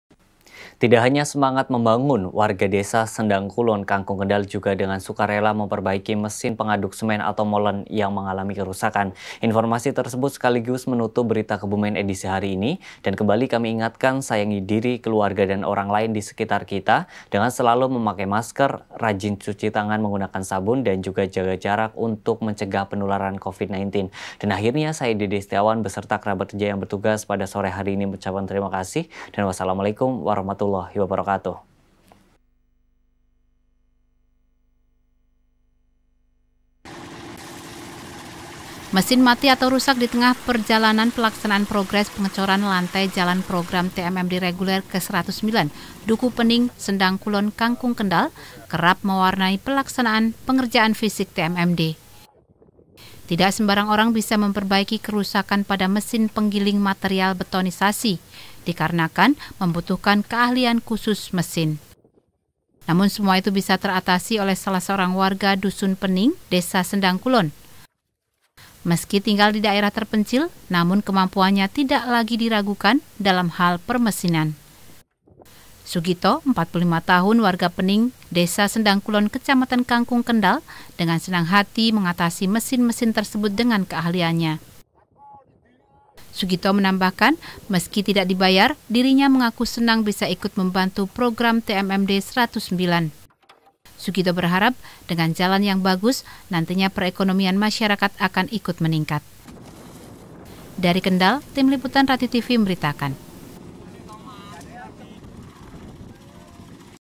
Buletin TMMD Kendal - Warga Rela Perbaiki Mesin Molen Tanpa Dibayar
Berita Kebumen Edisi Kamis, 15 Oktober 2020

Tidak hanya semangat membangun, warga Desa Sendangkulon Kangkung Kendal juga dengan sukarela memperbaiki mesin pengaduk semen atau molen yang mengalami kerusakan.